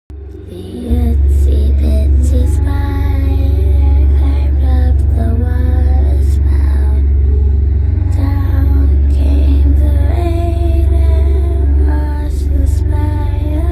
🕷 THE BLACK WIDOW SPIDER 🕷
I came across it while folding up the garden center plant table mats at the work house. I'm glad I saw it before it got me. 😨 🕷